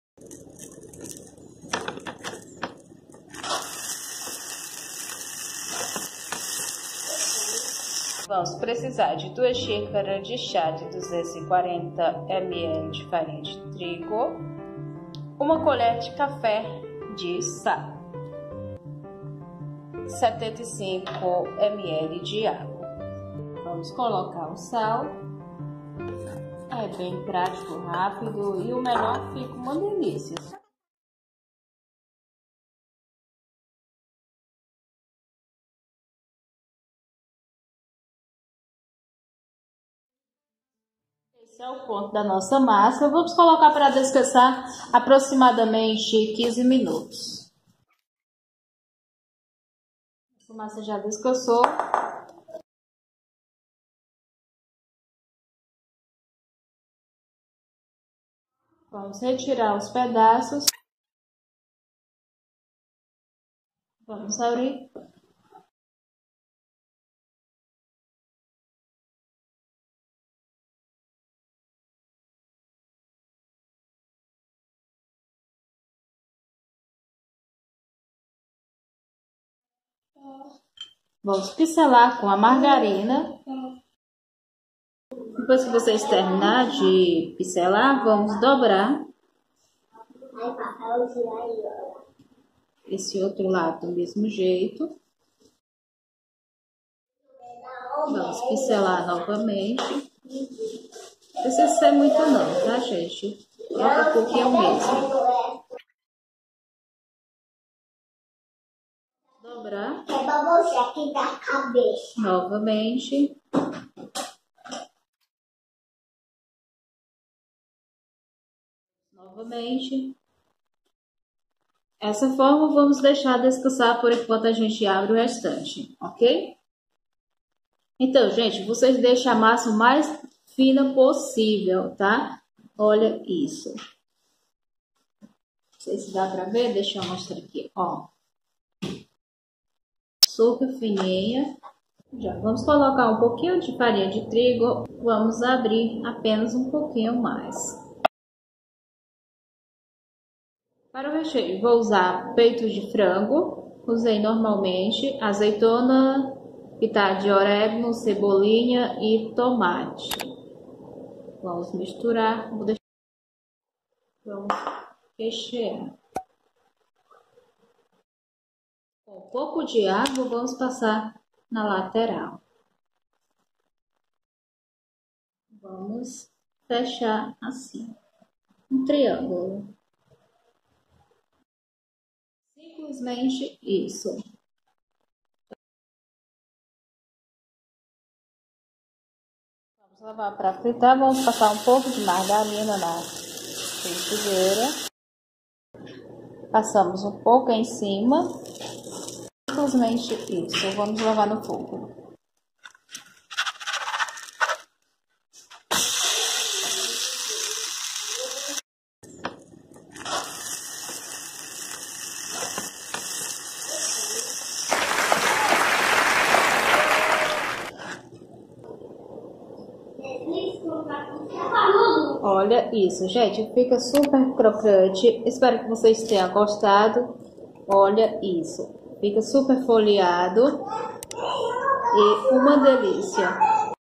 SEM ÓLEO SALGADO/Recheio Frango Ver e saborear mais fácil,Crocante👌👌
Todos Bem-Vindo!
ingredientes

 240ml de farinha de trigo 2 xícaras
1colher de café de sal
 para pincelar
manteiga ou margarina.